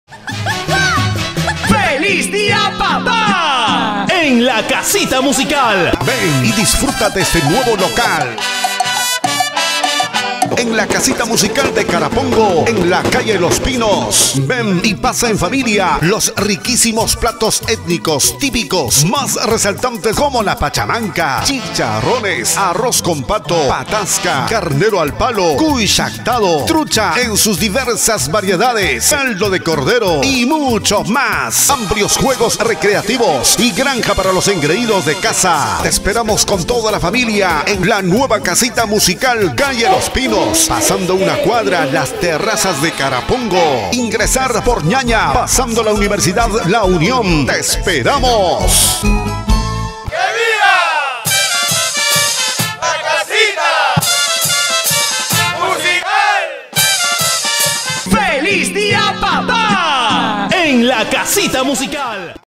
HAYDEE RAYMUNDO INVITA A LA CASITA MUSICAL EN EL DIA DE PAPA
VEN Y DESCUBRE LA NUEVA CASITA MUSICAL EN LA CALLE LOS PINOS, PASANDO LAS TERRAZAS DE CARAPONGO.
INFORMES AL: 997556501